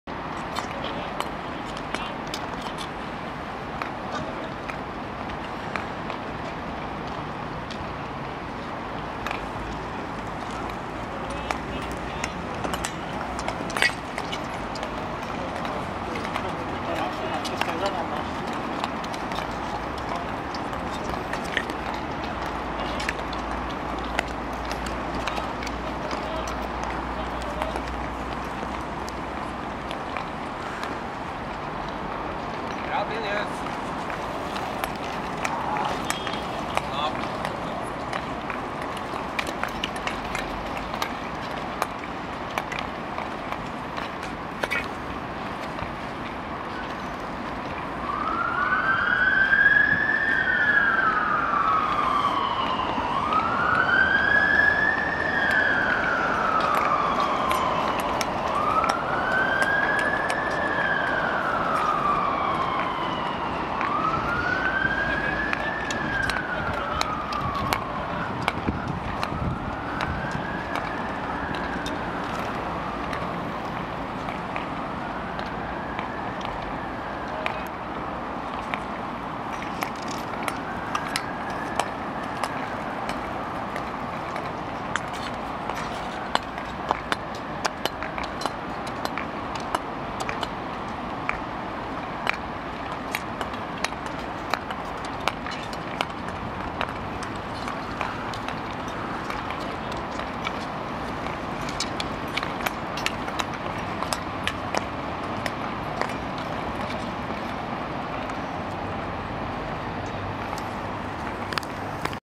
Bergbanen 27/10-16
Aktiv skøyteklubb trener på Bergbanen en uke før isen er ferdig på Valle Hovin